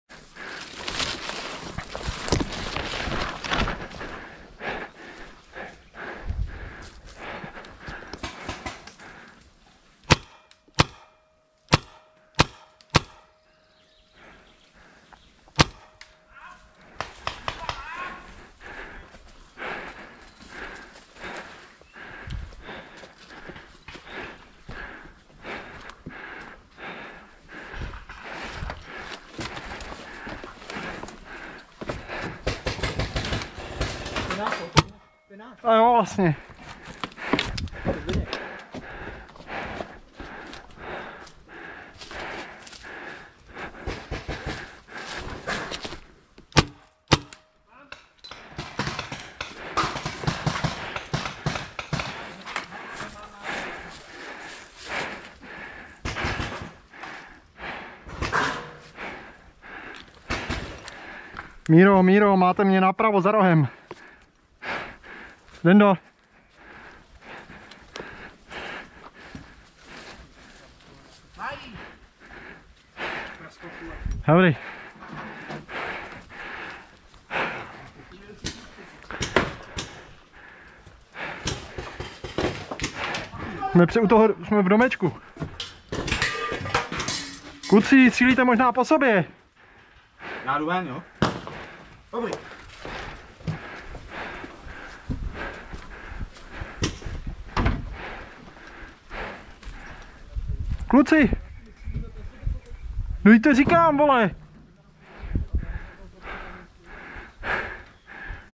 MagFed, Military Paintball, Gotcha - CZECH, Střelba do vlastních 2, 31.5.2020, GoPro 8
Občas se všichni v týmu necháme unést v zápalu hry.

Military Paintball, Bratronické spoďáry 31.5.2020
GoPro HERO8 Black
FirstStrike, Tiberius T15